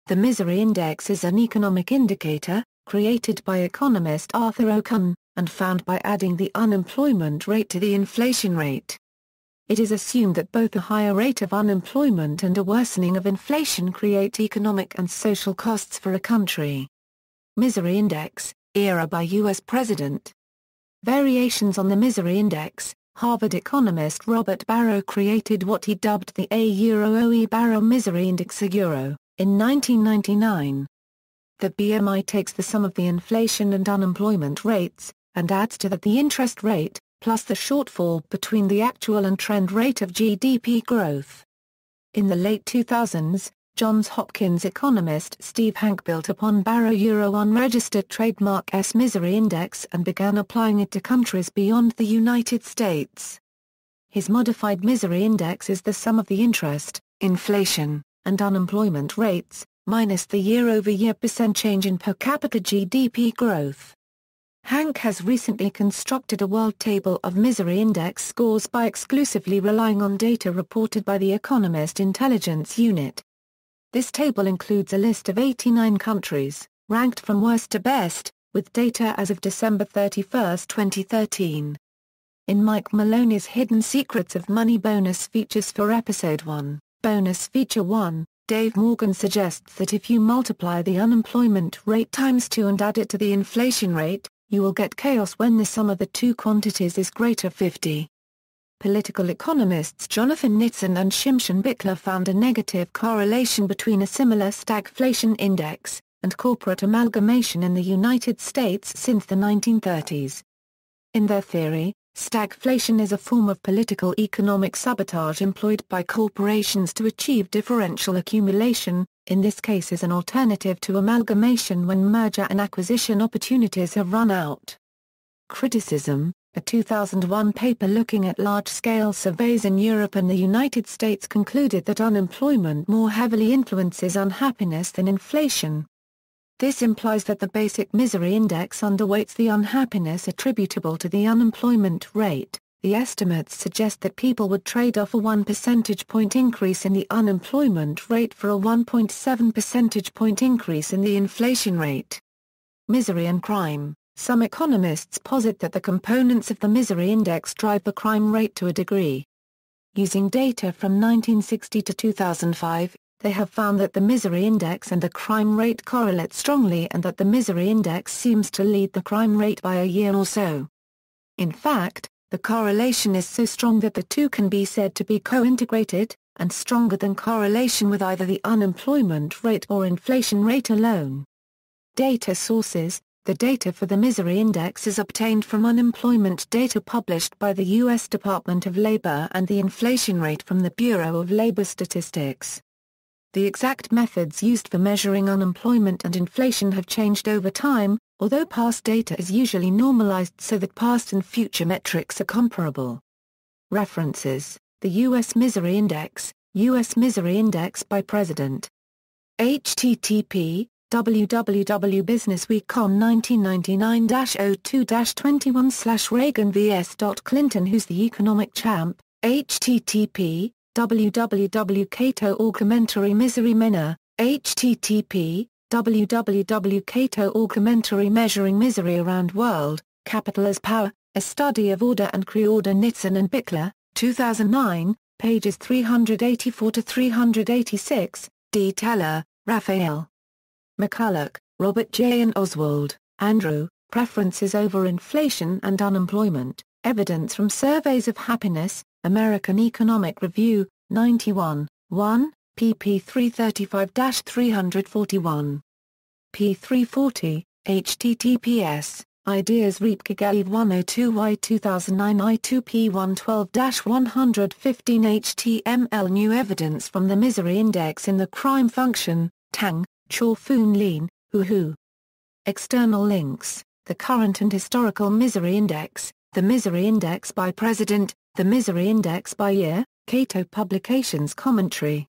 Misery index (economics)
The misery index is an economic indicator, created by economist Arthur Okun, and found by adding the unemployment rate to the inflation rate. It is assumed that both a higher rate of unemployment and a worsening of inflation create economic and social costs for a country.